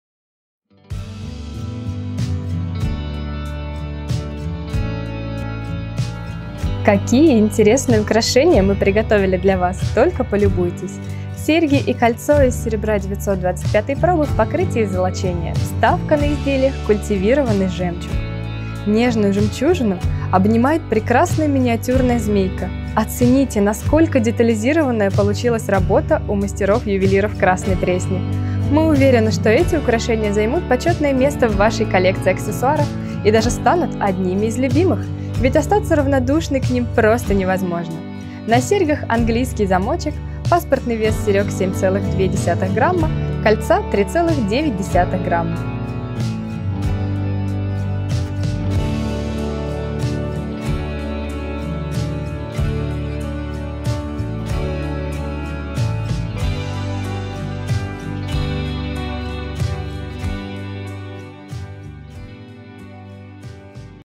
Серьги 33312285 и Кольцо 23312285 из серебра 925 пробы
Какие интересные украшения мы для вас приготовили - вы только полюбуйтесь! Нежную жемчужину обнимает прекрасная миниатюрная змейка! Оцените, насколько детализированная работа мастеров-ювелиров!  Мы уверены, что эти украшения займут почётное место в вашей коллекции аксессуаров и даже станут одними из любимых, ведь остаться равнодушной к ним просто невозможно! На серьгах классический английский замочек.

Паспортный вес серёг - 7,2 грамм, кольца - 3,9 грамма